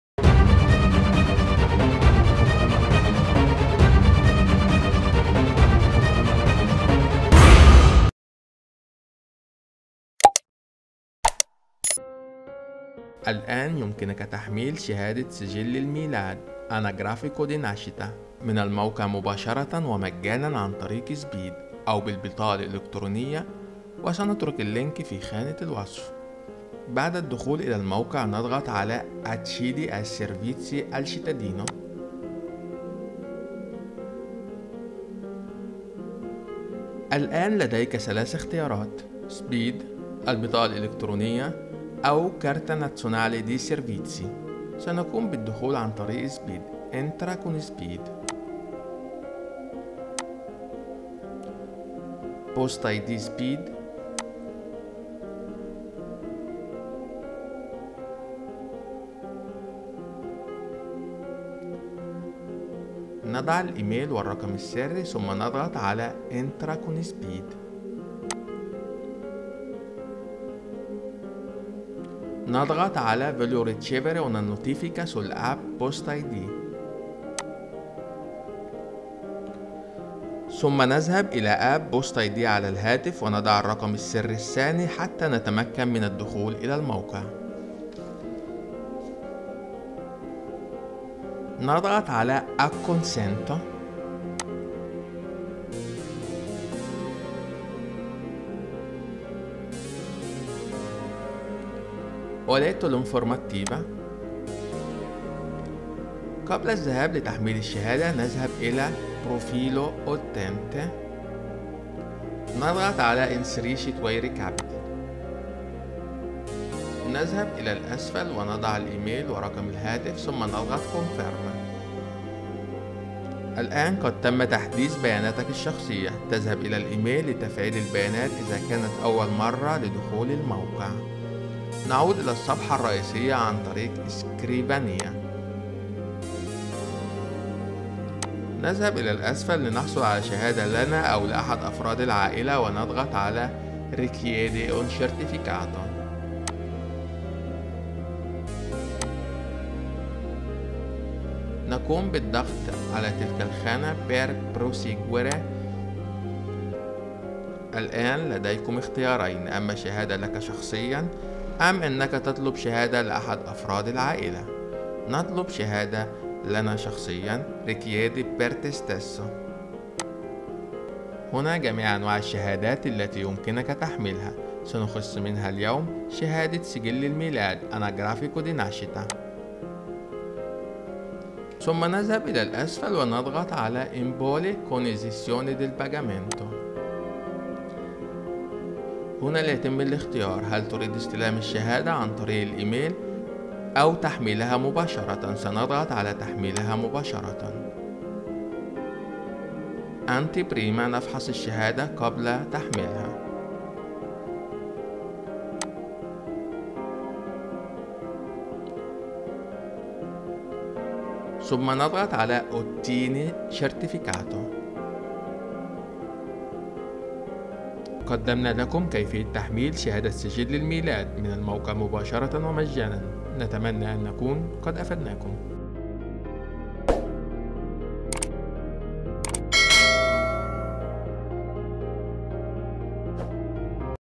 كيفية تحميل شهادة سجل الميلاد؟ ( ANAGRAFICO DI NASCITA )
خدمة جديدة لتسهيل الحصول على شهادات البيانات الشخصية و منها  تحميل شهادة سجل الميلاد , يمكنك تحميل الشهادة الان و مجانا

يمكنكم مشاهدة ايضا